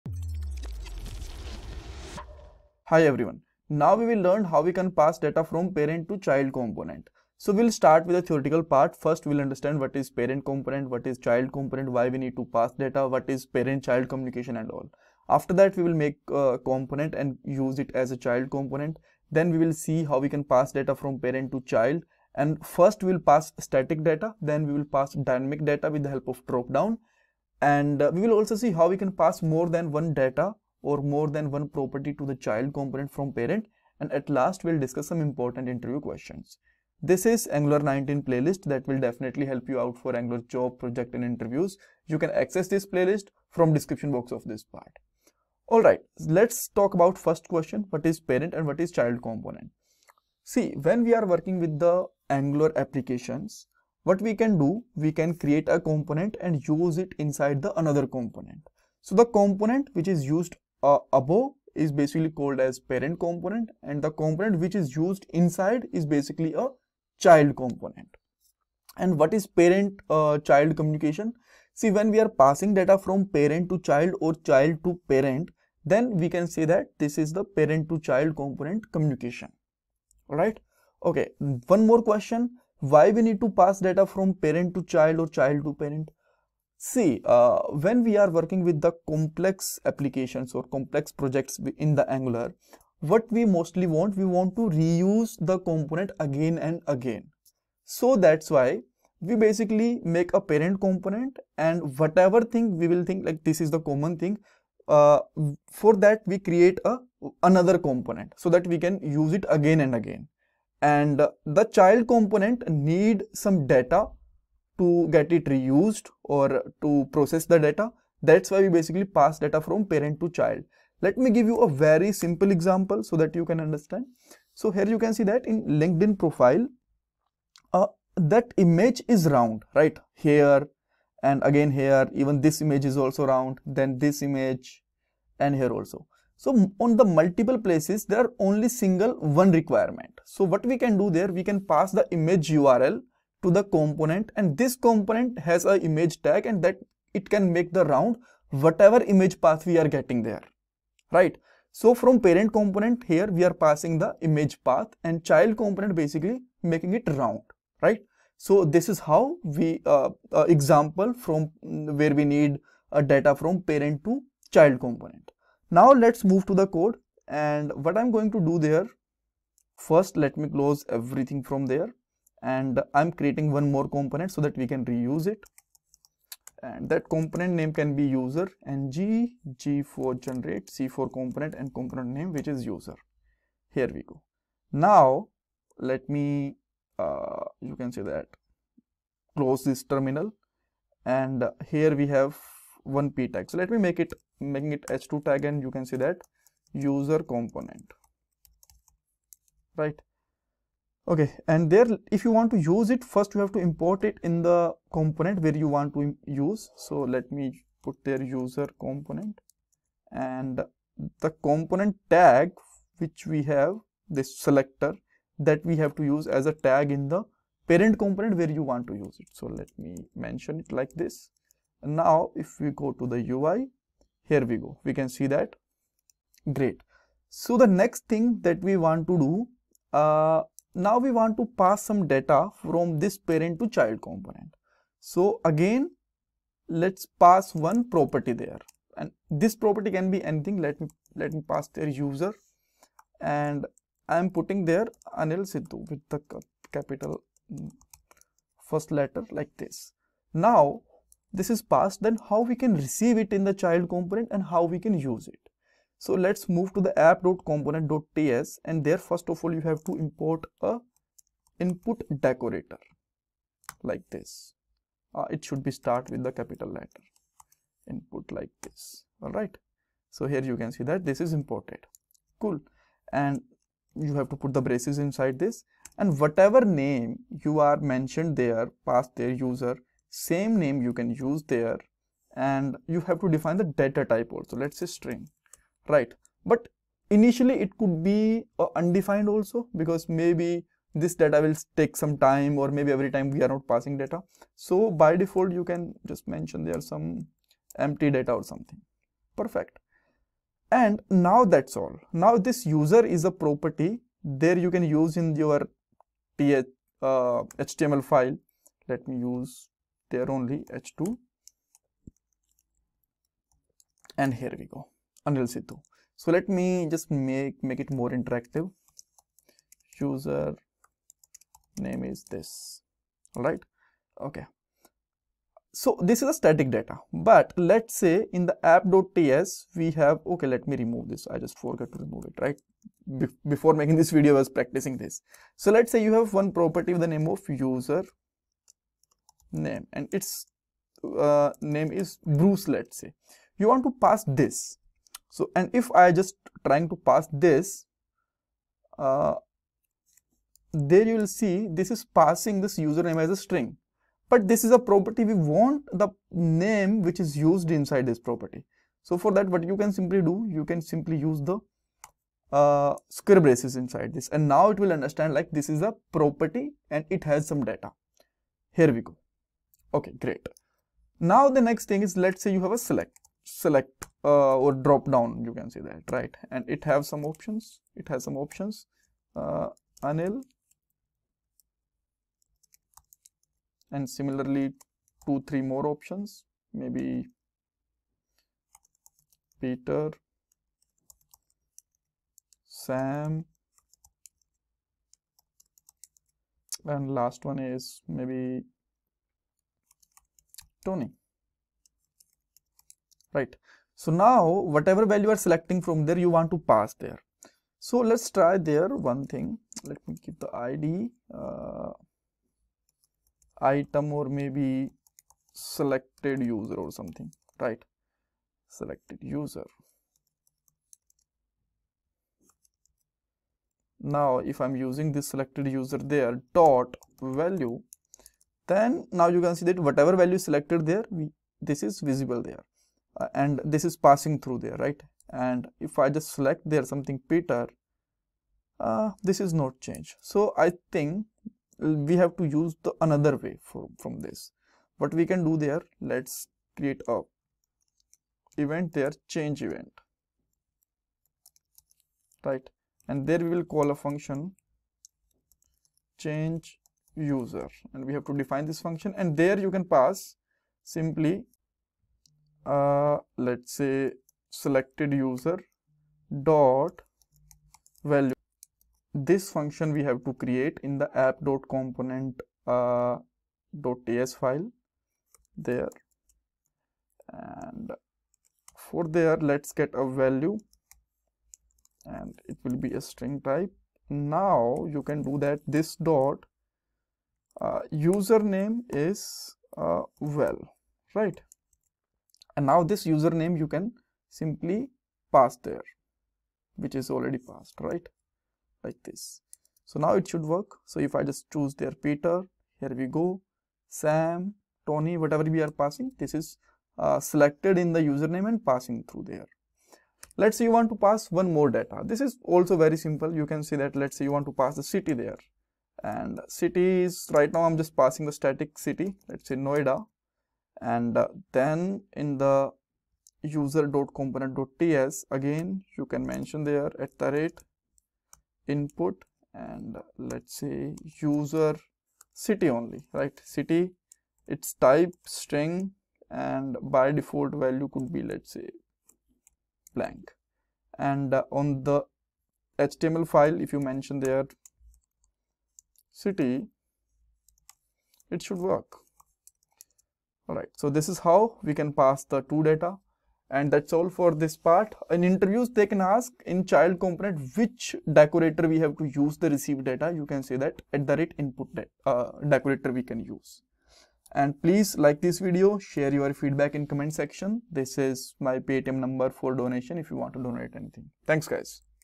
Angular 19 Tutorial #41 Passing Data from Parent to Child Component
In this Angular 19 tutorial, we'll delve into the essential concept of passing data from a parent component to a child component. This fundamental technique is crucial for building well-structured and maintainable Angular applications.
This video made by anil sidhu
We'll cover the following key aspects:
@Input() Decorator: Learn how to use the @Input() decorator to define input properties in your child component. This allows the parent component to pass data into the child.
Data Binding: Explore different data binding techniques, such as property binding and event binding, to effectively pass data between components.
Component Interaction: Understand the parent-child relationship and how to effectively communicate data between these components.
Practical Example: We'll demonstrate these concepts with a practical example, building a simple application that showcases how to pass data from a parent component to a child component and display it within the child.
By the end of this tutorial, you'll have a solid understanding of how to pass data between components in Angular 19 and effectively manage data flow within your applications.

Key takeaways:

Learn how to use the @Input() decorator to define input properties in child components.
Understand the parent-child component relationship and how to effectively communicate data.
Gain practical experience by building a simple application that demonstrates data passing between components.
Watch this video to enhance your Angular 19 development skills and build more efficient and maintainable applications.

Why we need to pass data parent to child
Make and use child component
Pass data from parent to child component
Pass value with dropdown
Pass more than one data in child
Interview Questions